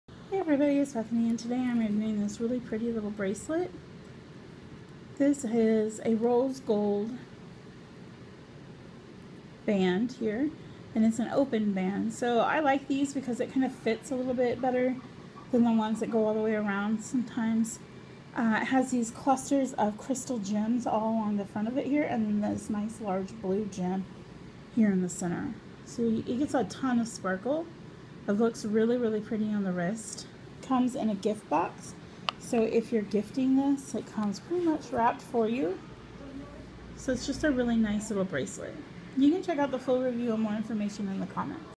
Star&Smile Blue Gem Cuff Bracelet
I just got this great bracelet from Star&Smile. This is a really pretty bracelet that has a gold band that is open. It's a great construction style has a solid piece that holds the gem and gives it a great stable backdrop and then transitions into a open cuff. The gems is very pretty and has a lot of sparkle Really pretty blue color. Nice oval shape. I love the style of it, and it goes with just about everything. Very modern design and really well made. Great bracelet! I received this item complimentary or at a reduced price for my honest review. However, I only recommend products I truly love and use to my readers.